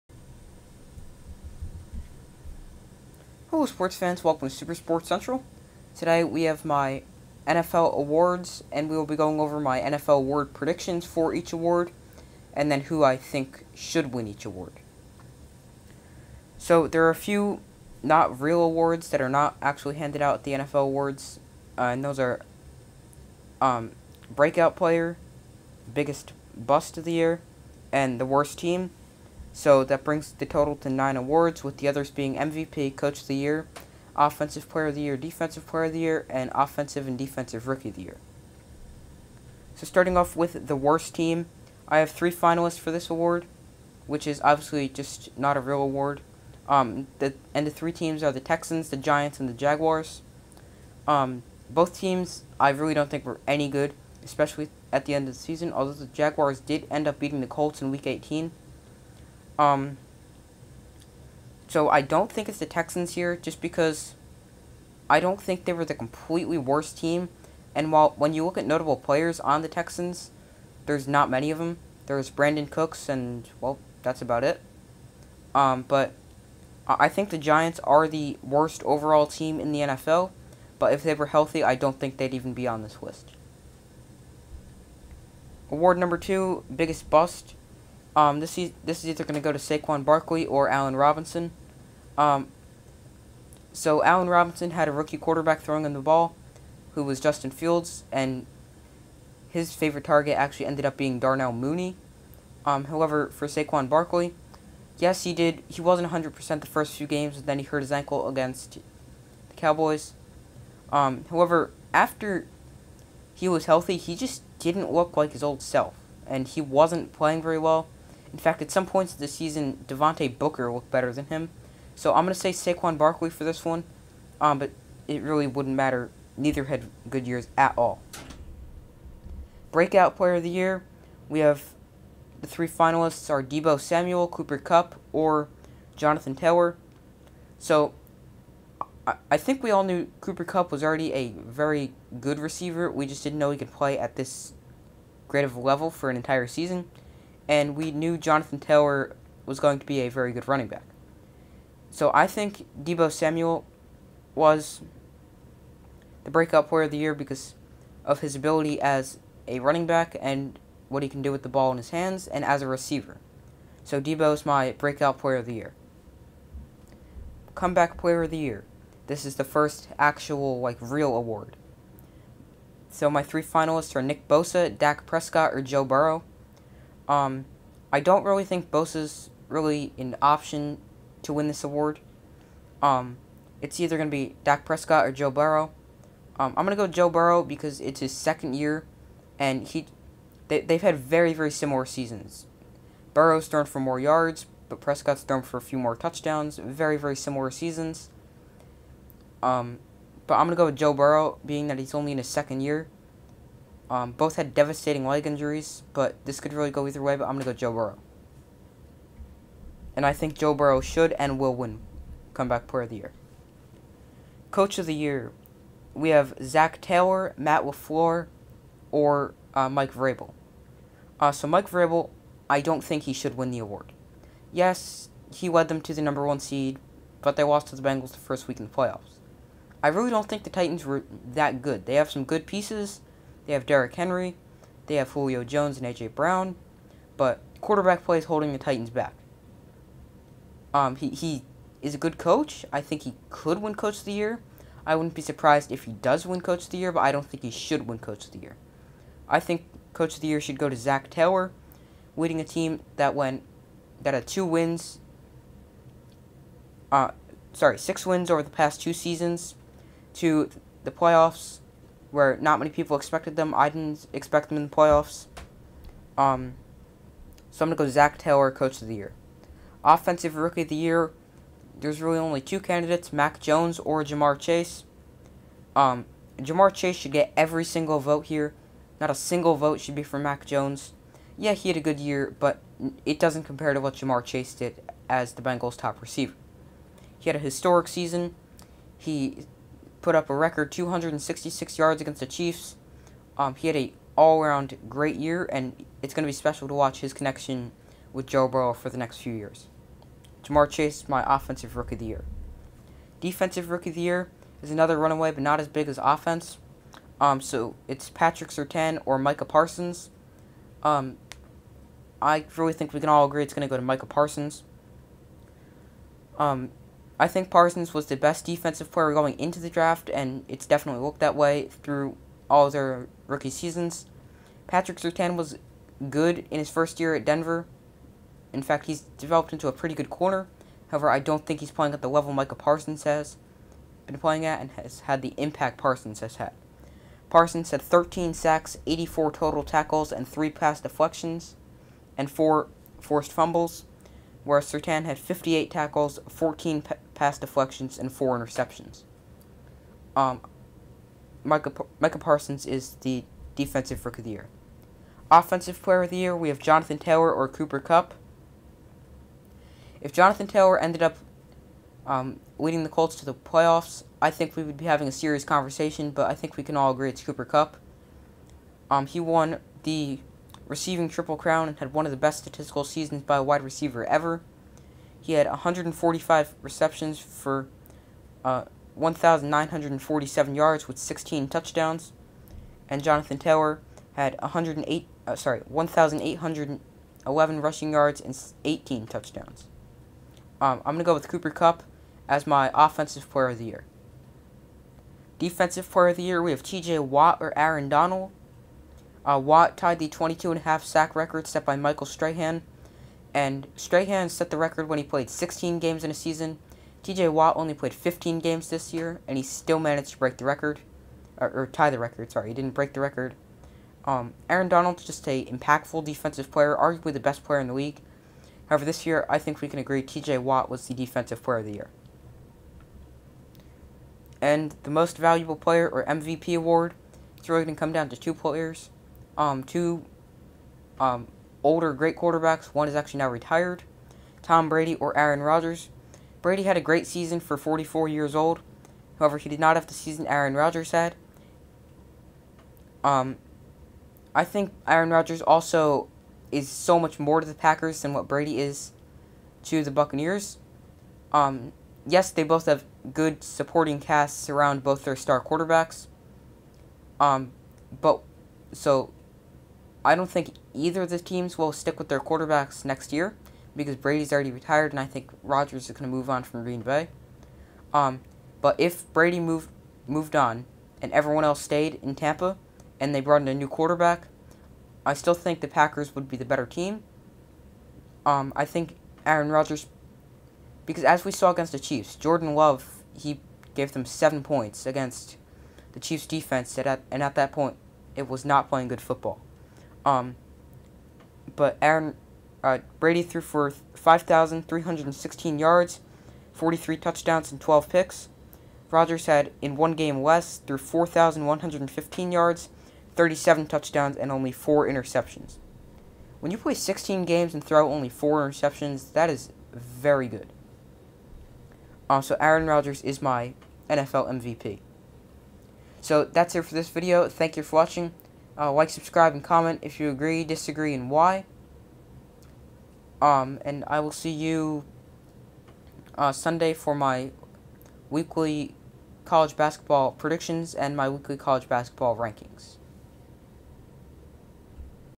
My 2021-2022 NFL Award Predictions
We will go over my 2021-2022 NFL award predictions, and who I think should win each award. We will go over my predictions for MVP, Offensive Player of the year, Defensive player of the year, offensive rookie of the year, defensive player of the year, comeback player of the year, coach of the year and more.